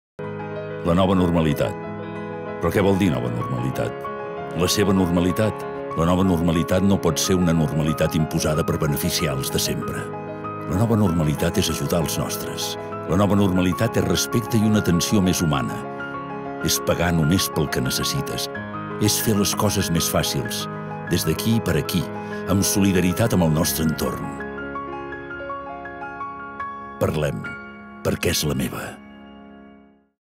La nova normalitat
La nova normalitat no pot ser una normalitat imposada per beneficiar els de sempre.

És ajudar als nostres. És el respecte i l'atenció més humana.

La nova normalitat és pagar només pel que necessites.

És fer les coses més fàcils. Des d'aquí, i per a aquí.